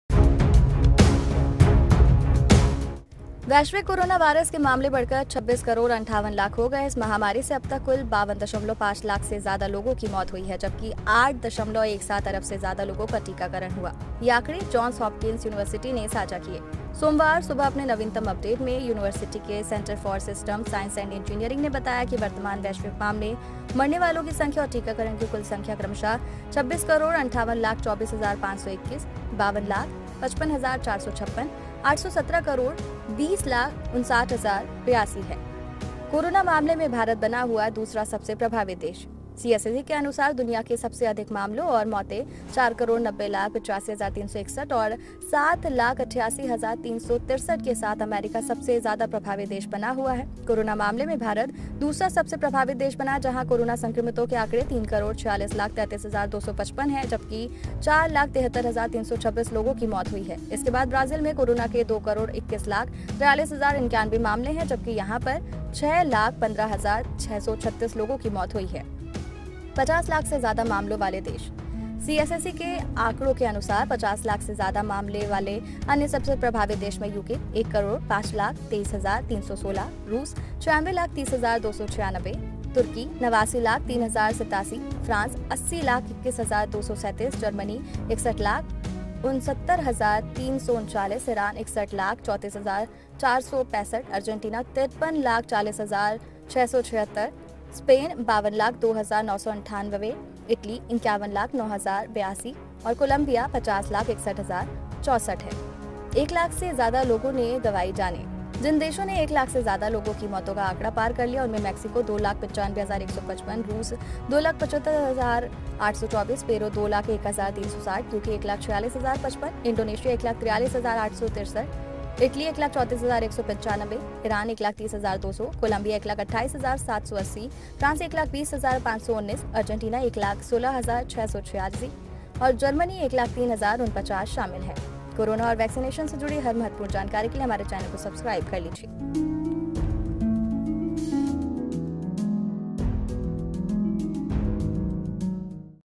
Corona World Update : दुनिया में तेज़ी से बढ़ने लगे कोरोना के मामले ! जानिए किस देश में क्या है हाल
Corona World Update: Corona Cases Started Increasing Rapidly In The World ! Know What's Going On In Which Country | Corona Virus And Corona Virus Vaccine All Latest News And Updates

EDITOR ►Shadik